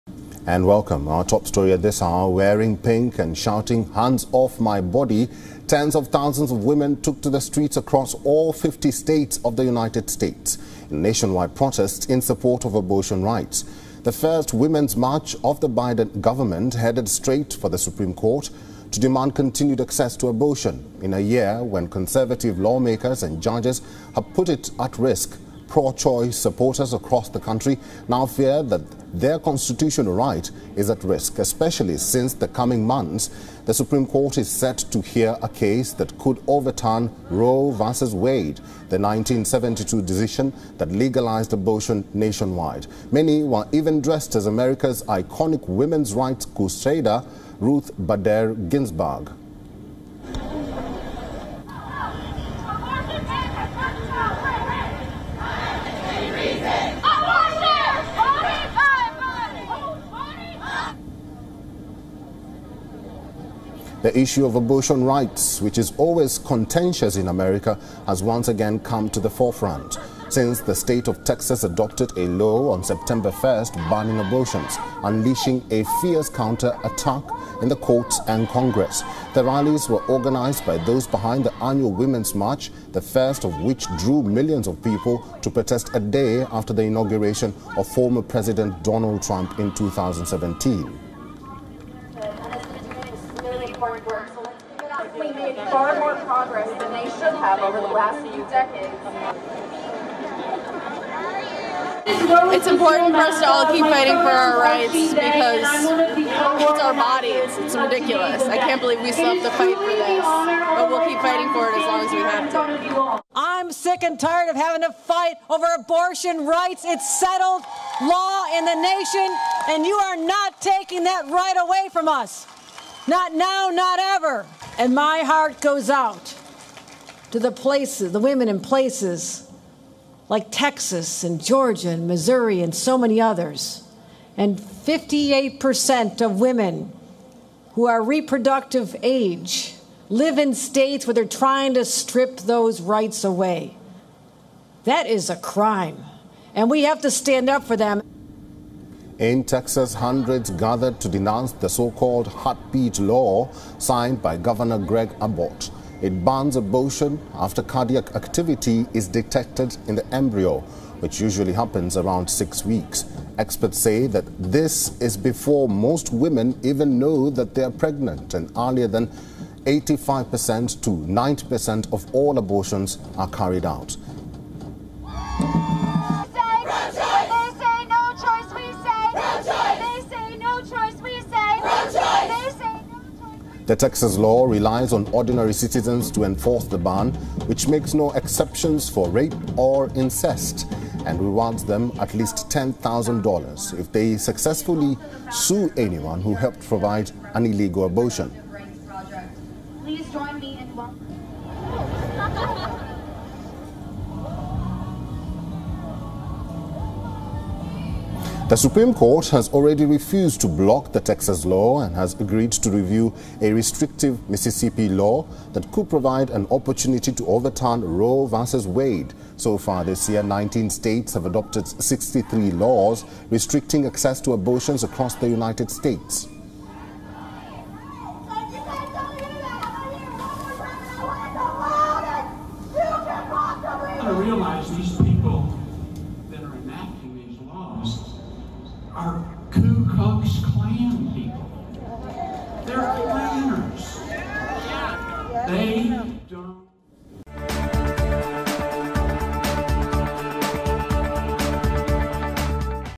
Rallies held across the US against abortion ban | WION News | Latest English News
Women marched by the thousands on Saturday on the Supreme Court, the Texas Capitol and cities across the United States to protest increasing state restrictions on abortion.